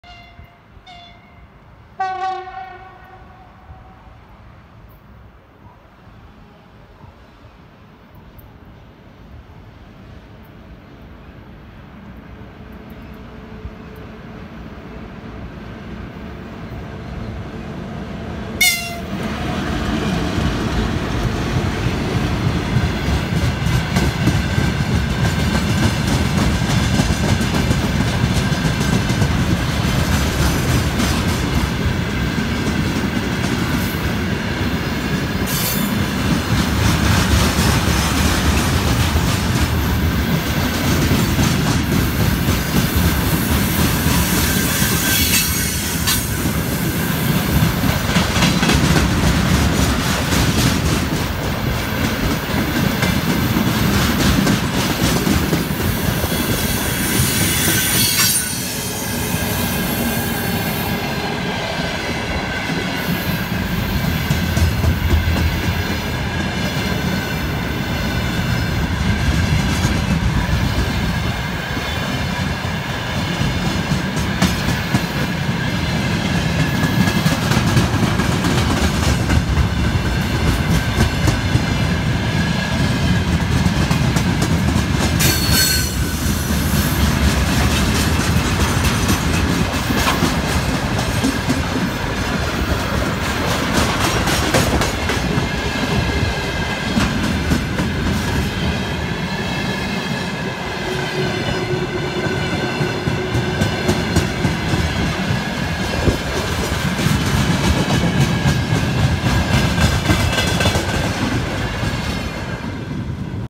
Электровоз ВЛ10У-642 с грузовым поездом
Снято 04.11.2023
Электровоз ВЛ10У-642 (приписан к ТЧЭ-5 Орехово Московская ЖД) с грузовым поездом на ст. Которосль, Северная ЖД
Россия, Ярославль